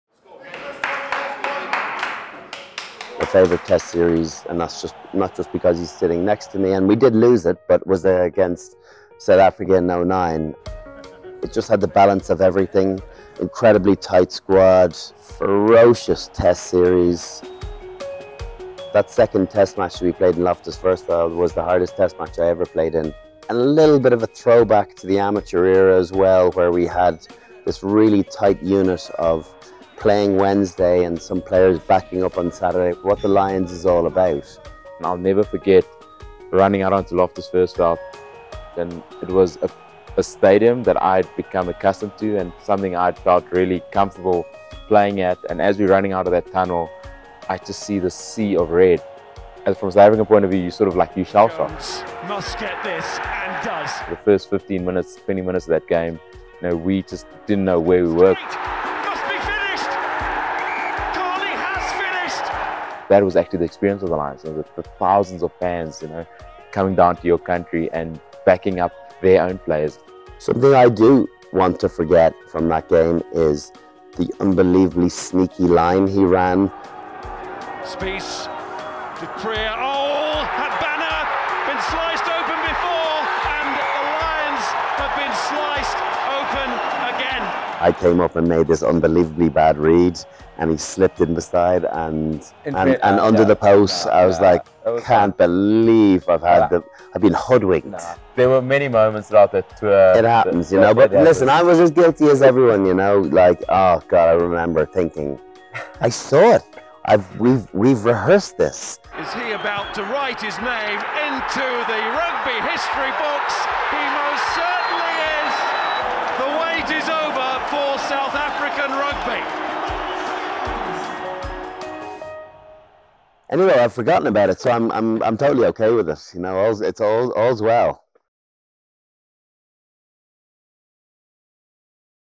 O'Driscoll and Habana on the 2009 Tour | Laureus Series
Brian O'Driscoll and Bryan Habana sat down and talked through their favourite moments from the 2009 Tour of South Africa.